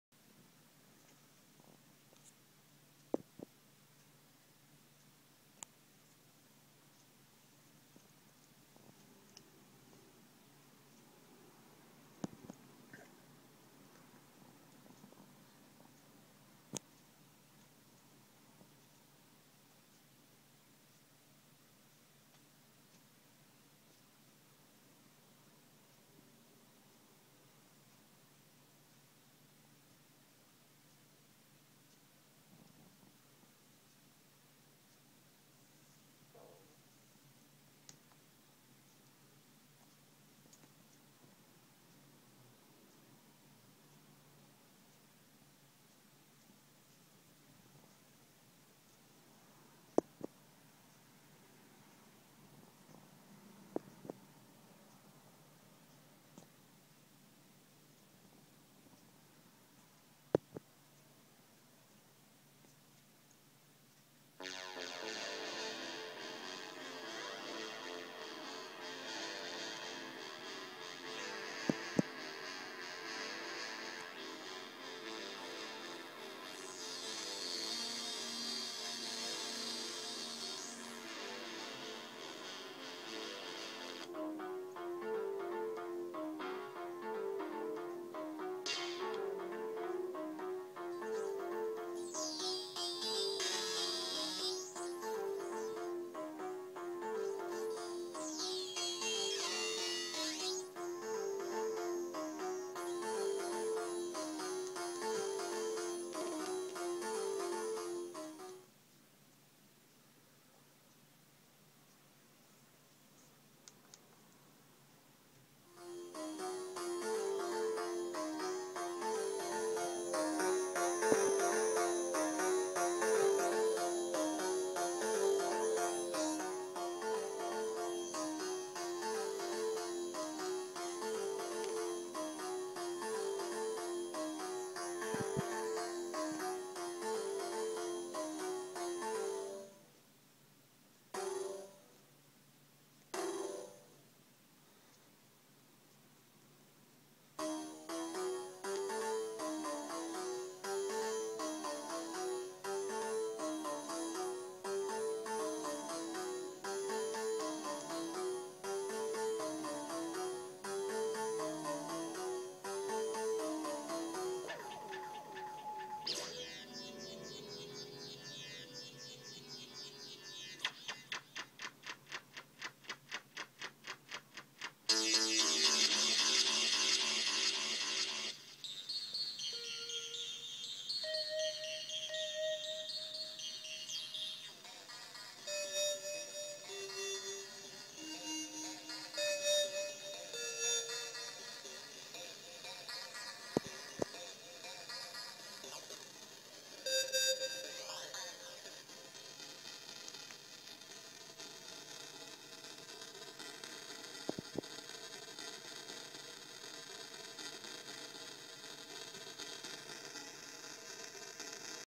Animoog + Genome
Hey guys, after I posted my last video MOOG put out an update which allowed for background audio support on Animoog which meant that video was outdated. So, here is an updated "how to" showing off Animoog's new features. Enjoy! Have a great day!!!